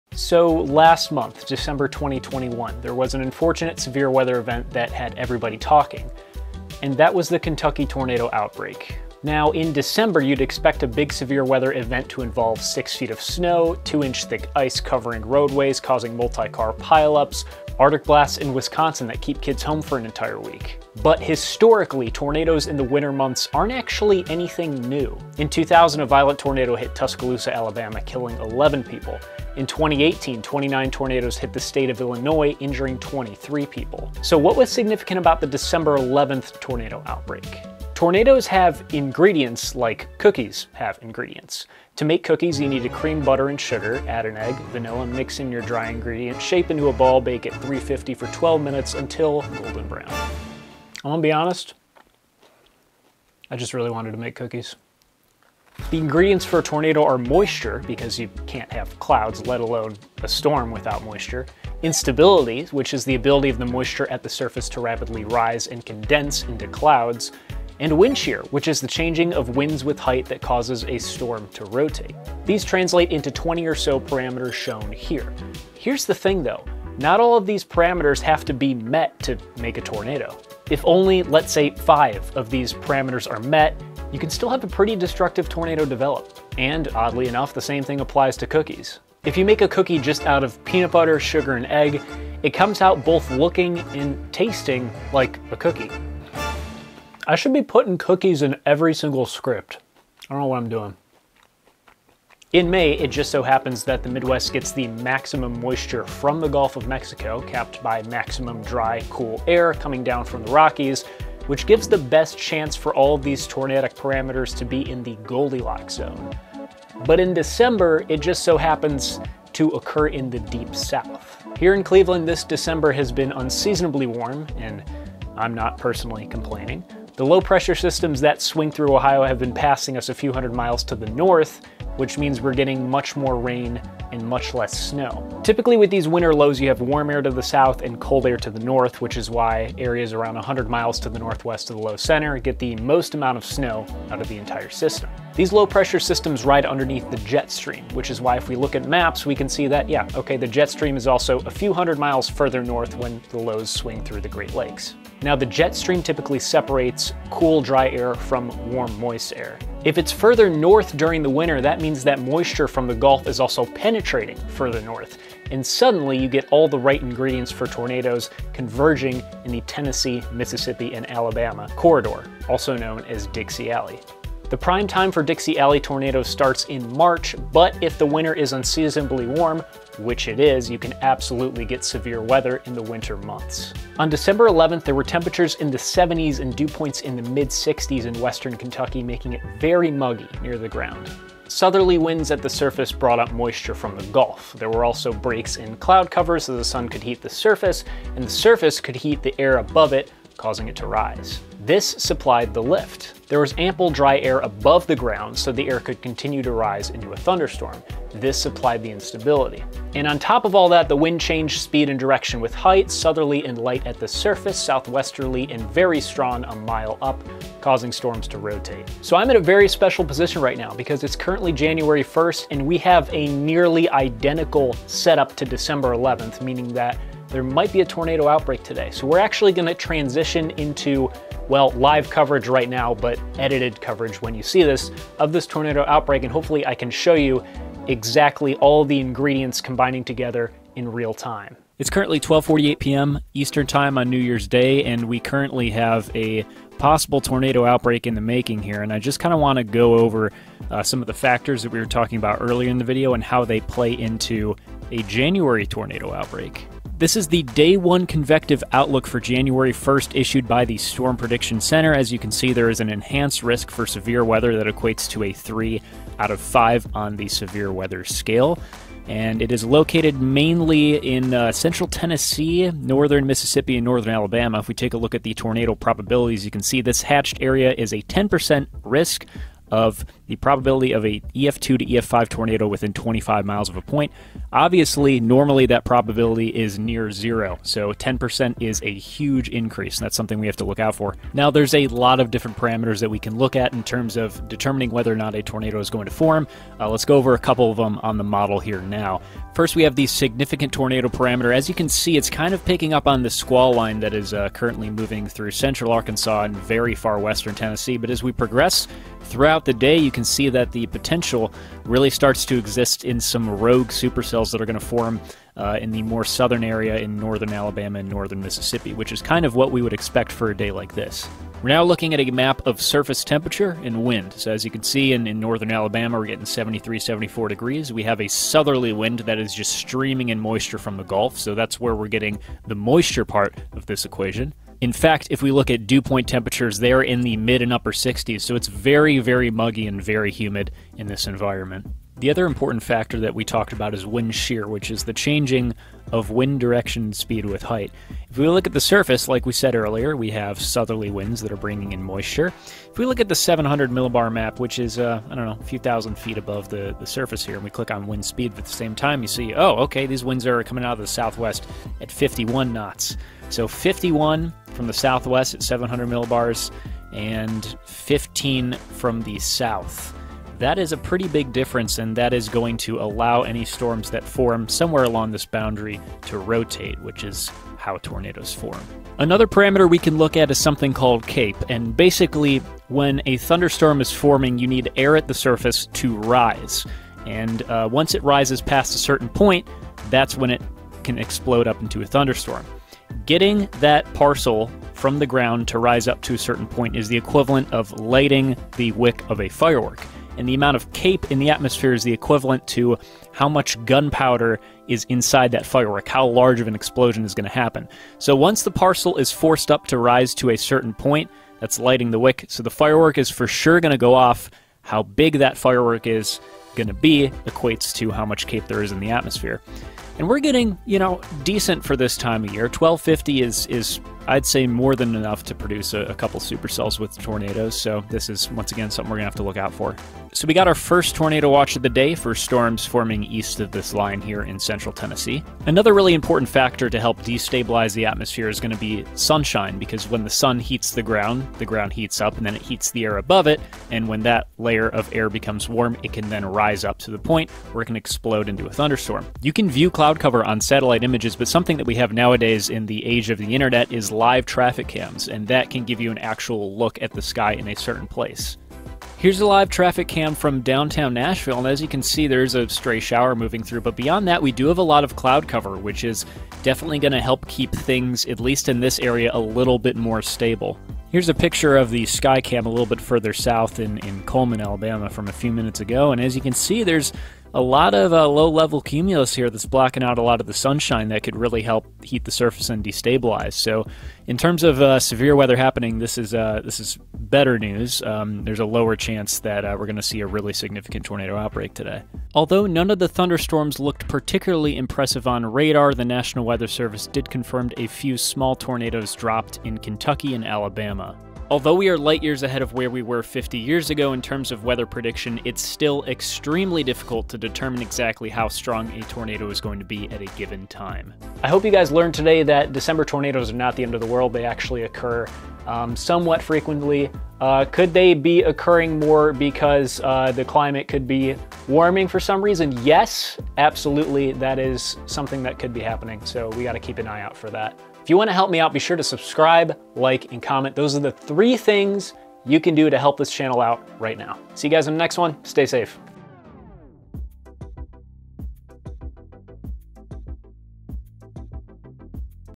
why December Tornadoes happen... sometimes.
Today we talk about what happened last month in Kentucky, as well as the dynamics of winter tornadoes in general.